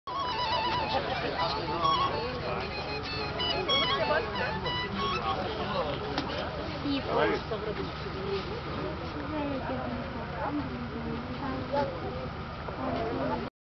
FATNAS SUNSET IN SIWA פטנס ,שקיעה בסיווה
נסענו עם האופניים ביום קריר לראות את השקיעה מעל הנווה מדבר באמצע המדבר ,התאגדו מסביב מיצרים ועוד קומץ קטן מאוד של תיירים שבאו לראות את השקיעה המרהיבה על המקום .